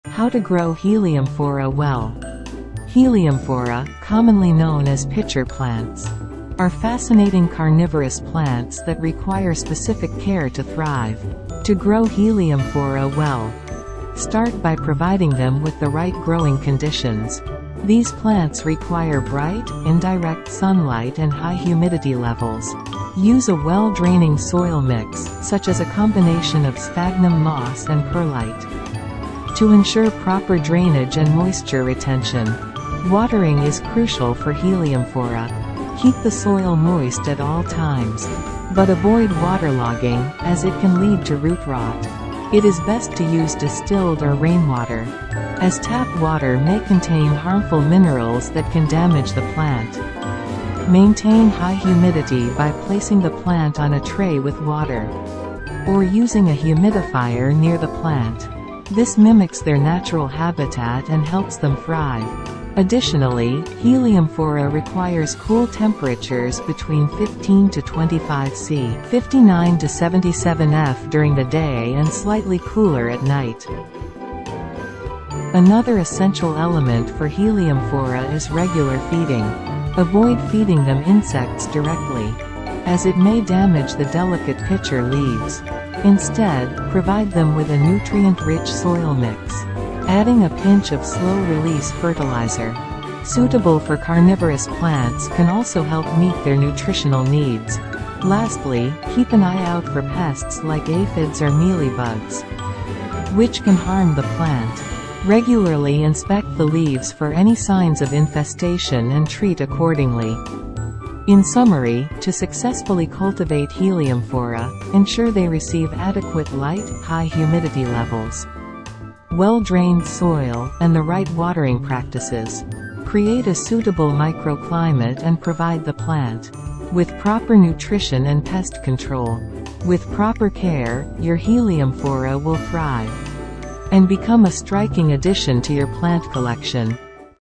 Heliamphora Care for Beginners
Carnivorous plants heliamphora care for beginners. This video is intended as a starting point for those who want to grow carnivorous plants, showing how to care for them, how to replant them, and how carnivorous plants change. This channel will be useful for those who want to grow carnivorous plants and for those who have been growing carnivorous plants for years.